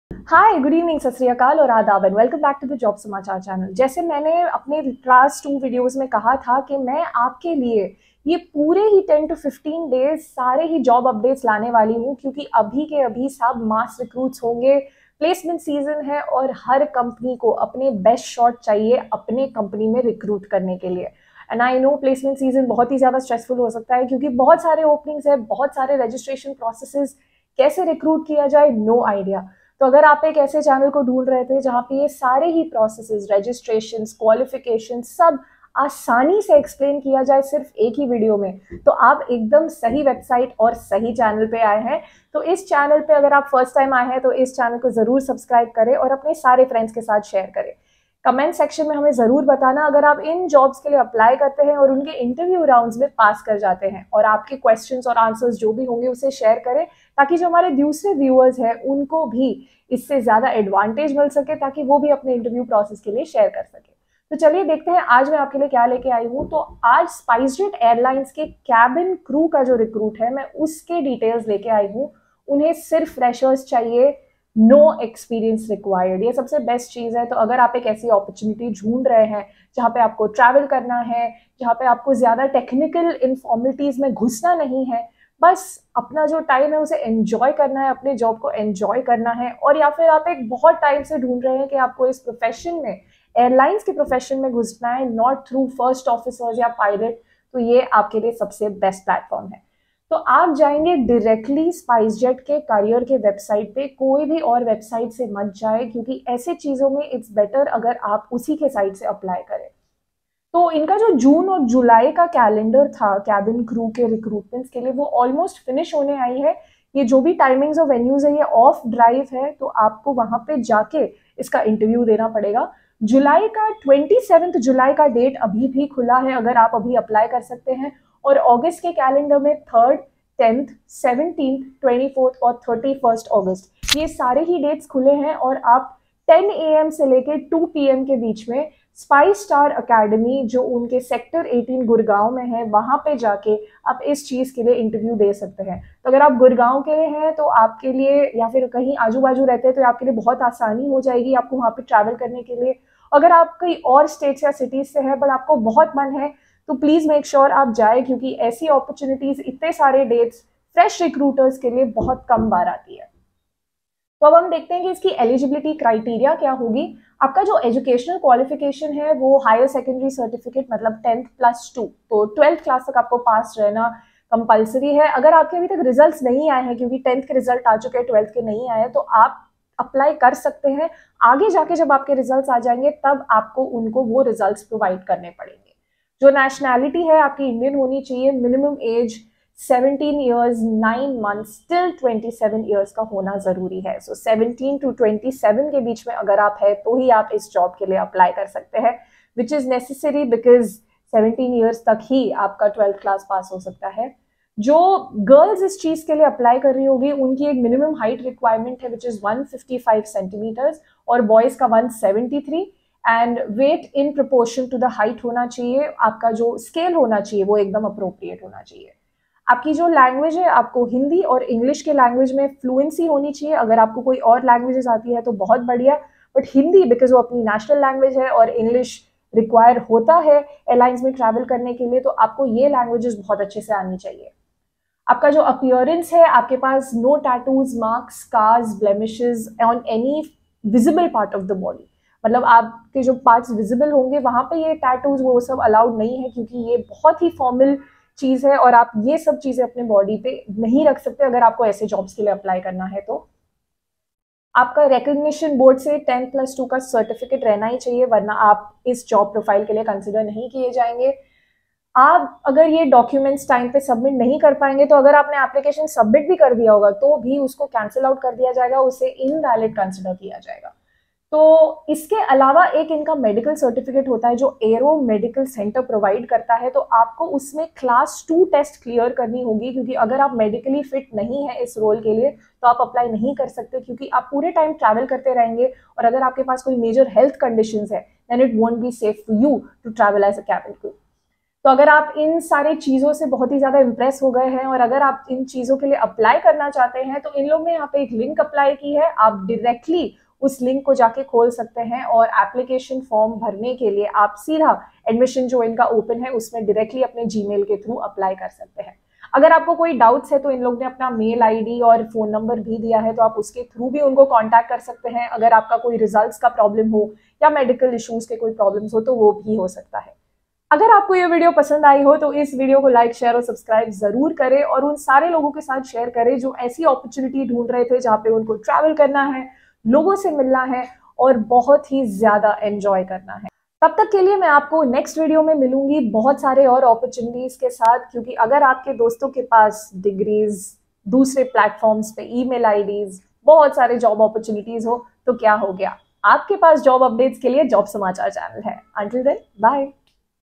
SPICEJET HIRING | CABIN CREW 2022 | APPLY NOW
Welcome back to the Jobs Samachar Channel!
Aaj ke placement Drive video ke liye we have got cabin crew recruitments from Spicejet Airlines!

LINKS TO APPLY and REGISTER

0:00 Introduction
1:06 Spicejet cabin crew role calendar
2:49 Qualifications and registration links